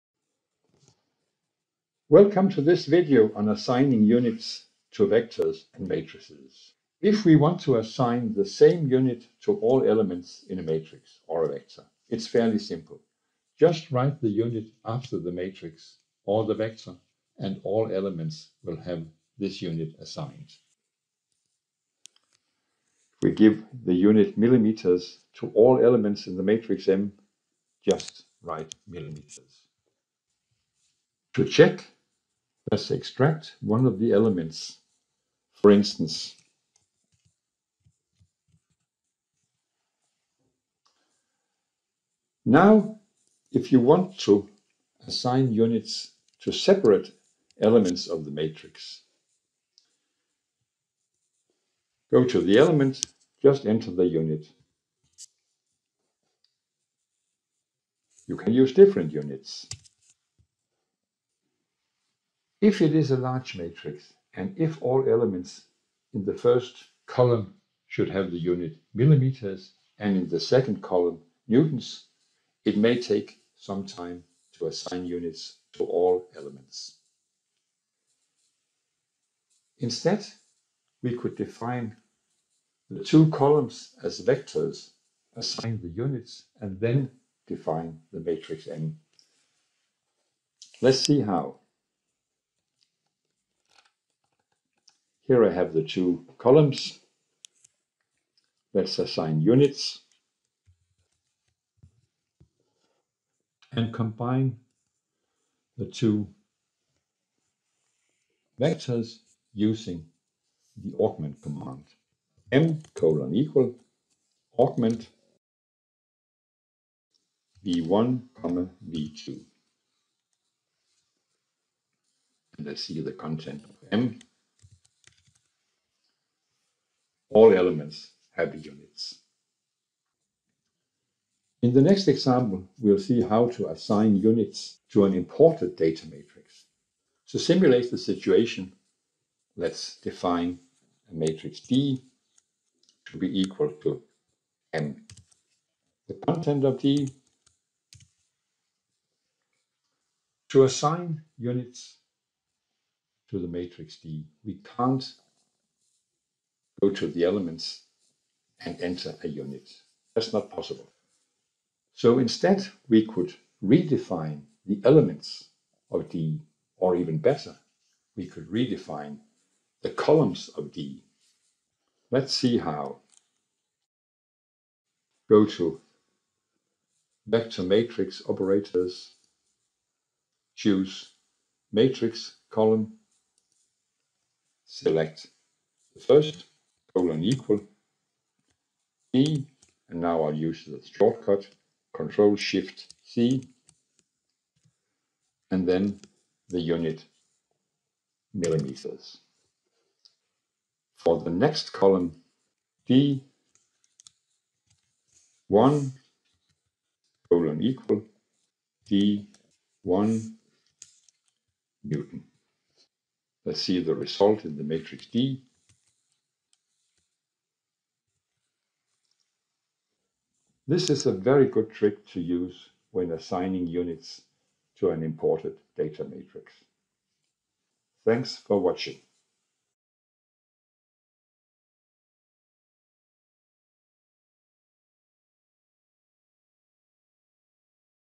Assigning Units to Vectors and Matrices in Mathcad Prime
Watch this video to learn shortcuts and tips on assigning units to a definition of a vector or matrix, as well as assigning units to an imported data matrix. These include assigning units to every value in a matrix all at once, assigning a unit to each value in a matrix individually, and assigning all values within a certain column with a certain unit.

Assigning units to values and performing calculations with unit balancing and intelligence is a key capability in PTC Mathcad Prime.